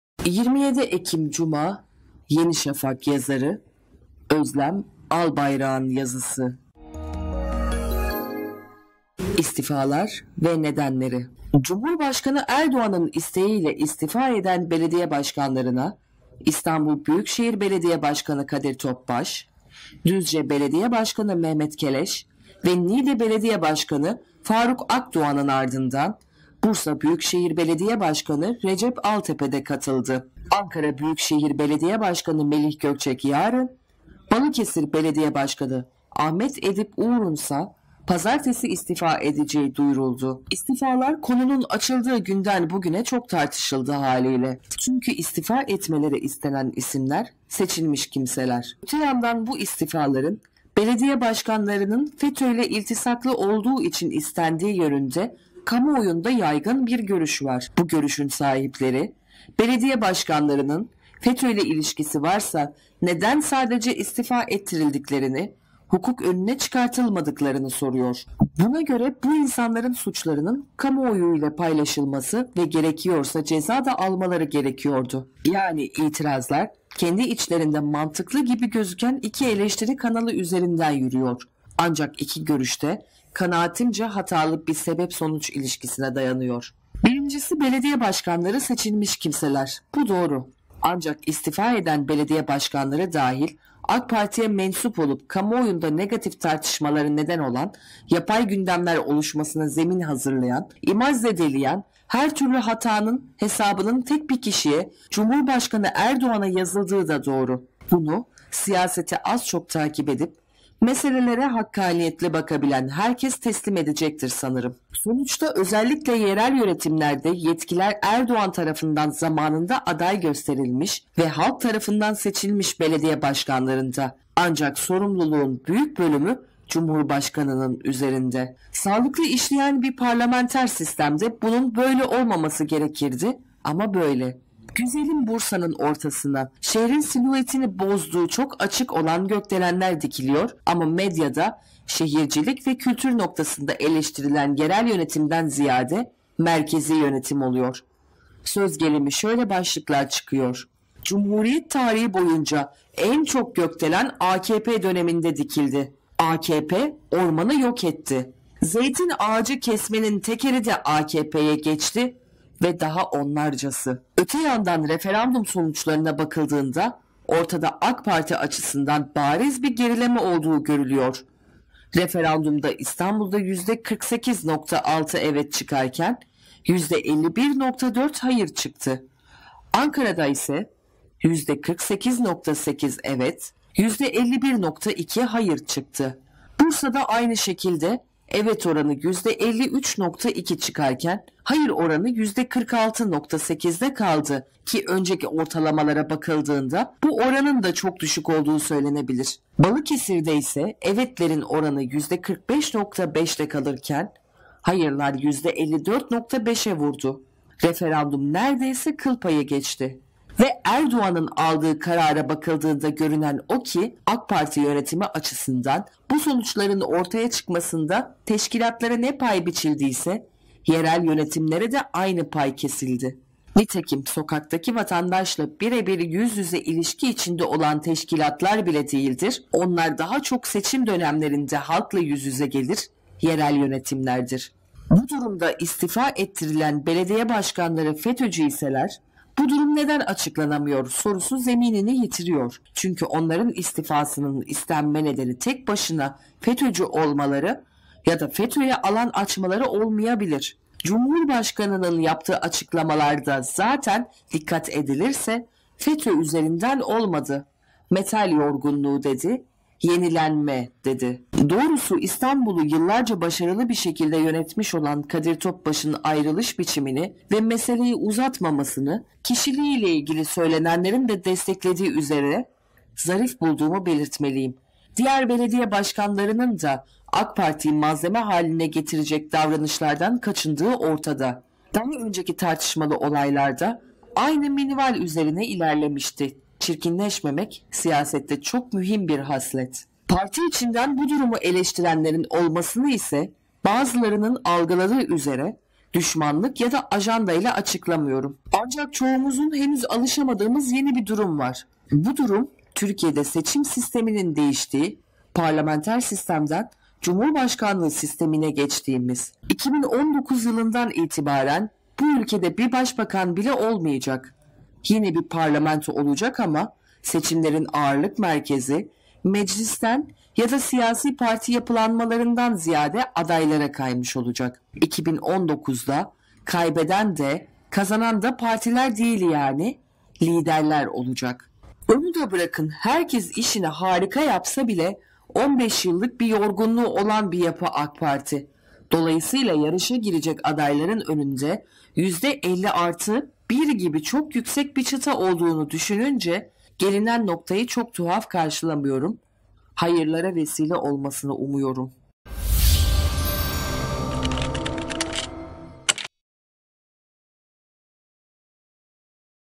İstifalar ve nedenleri - Özlem Albayrak
Cumhurbaşkanı Erdoğan'ın isteğiyle istifa eden belediye başkanlarına İstanbul Büyükşehir Belediye Başkanı Kadir Topbaş, Düzce Belediye Başkanı Mehmet Keleş ve Niğde Belediye Başkanı Faruk Akdoğan'ın ardından, Bursa Büyükşehir Belediye Başkanı Recep Altepe de katıldı. Ankara Büyükşehir Belediye Başkanı Melih Gökçek yarın, Balıkesir Belediye Başkanı Ahmet Edip Uğur'un ise pazartesi istifa edeceği duyuruldu.